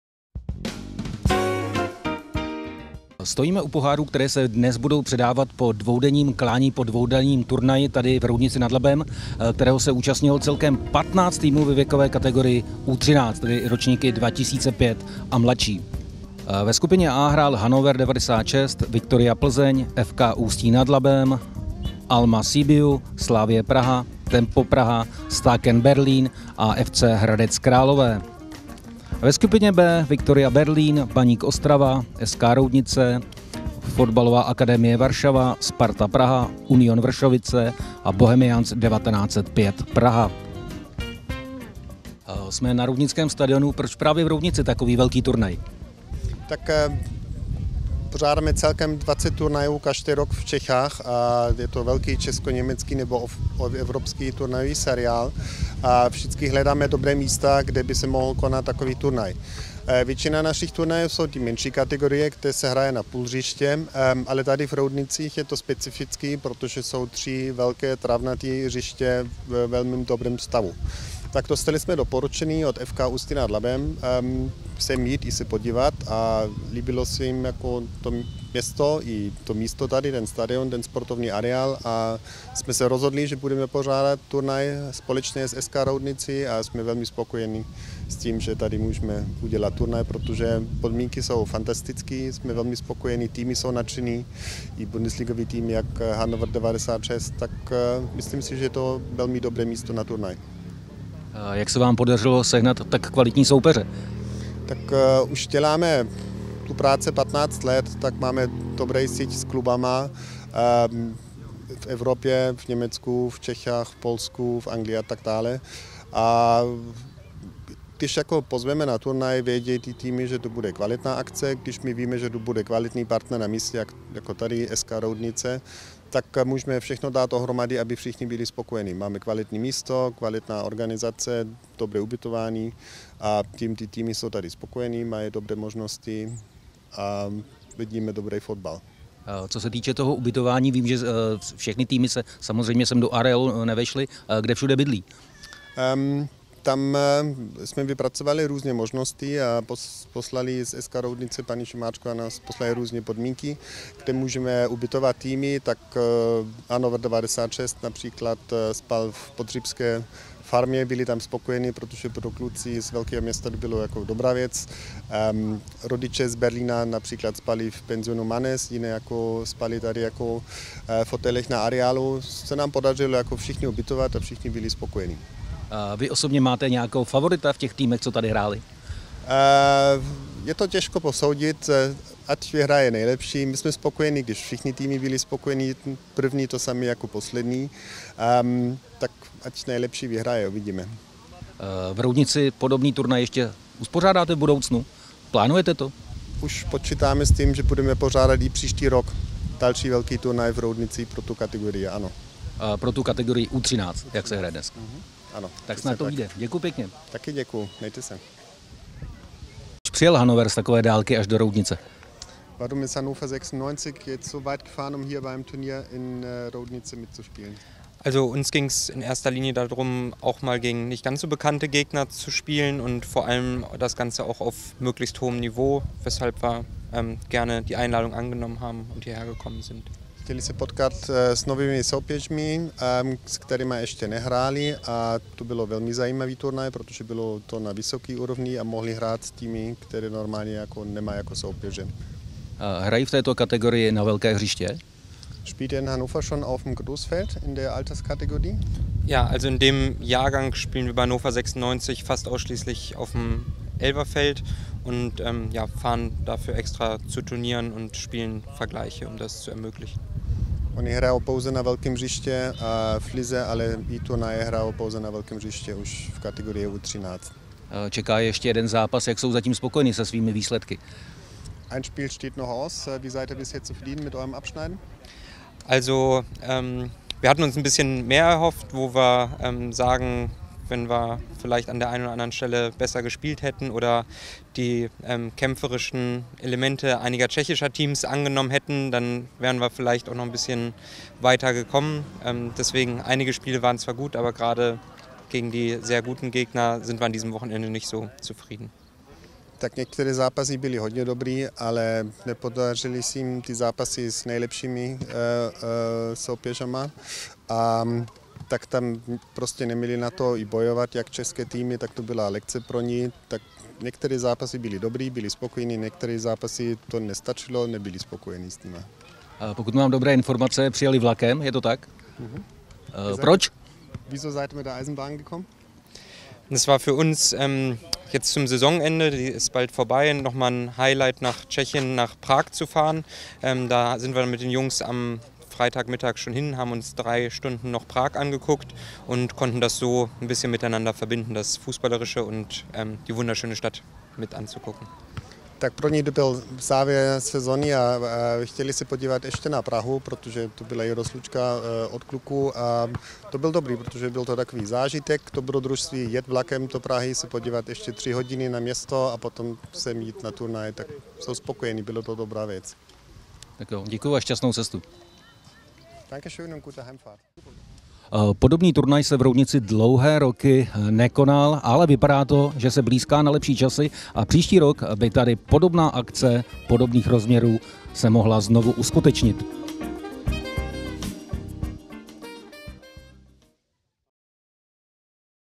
Mezinárodní fotbalový turnaj kategorie U13
15 týmů  přes víkend změřilo své síly na roudnickém trávníku.